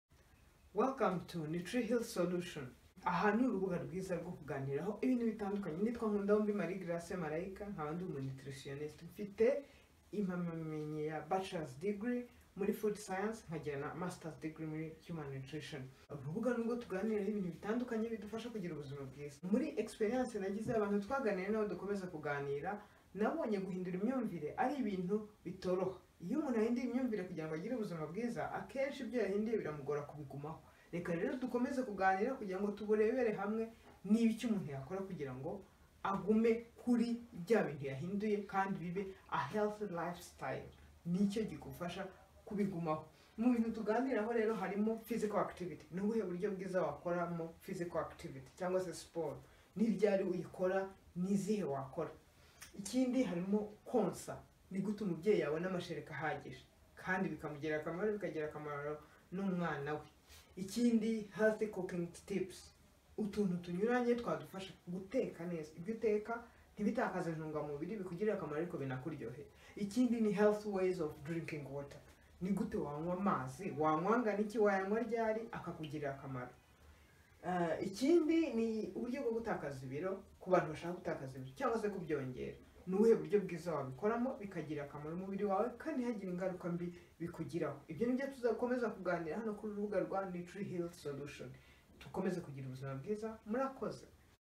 WELCOME TO NUTRIHEAL SOLUTION
Ikaze kuri channel tuganiriraho ibintu bidufasha gutera intambwe zituganisha kuri a healthy lifestyle.
 Murakoze dukomeze tugire ubuzima bwiza.
be blessed, let's love ourselves